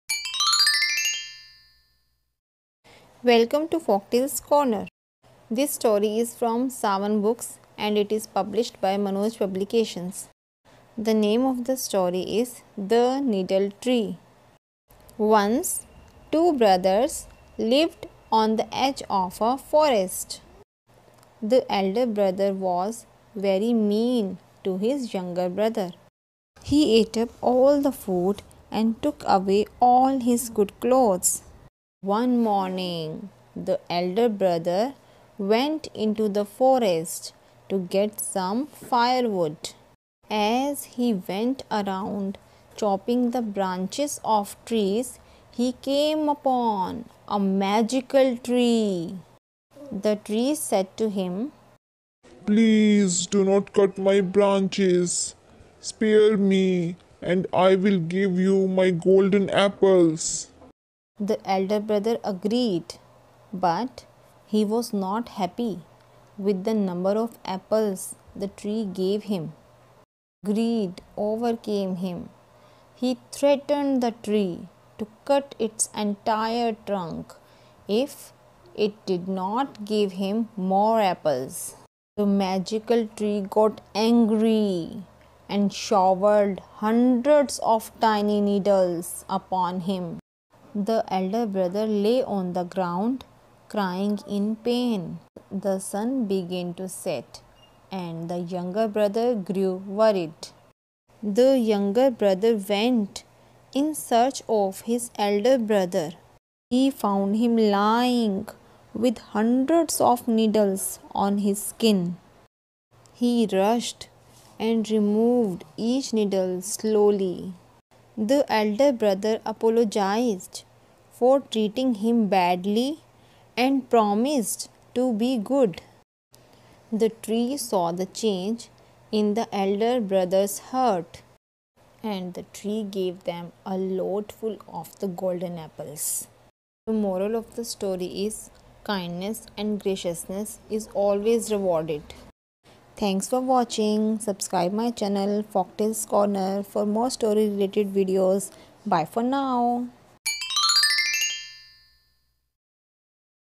The needle tree story for kids read aloud by Sawan books.. bedtime stories..for beginners also
बच्चों की कहानियां मजेदार कहानियाँ sawanbooks bedtimestories kids stories kidsstories kidsstories english englishstories english fluent#readaloud
corner#fairytales#viral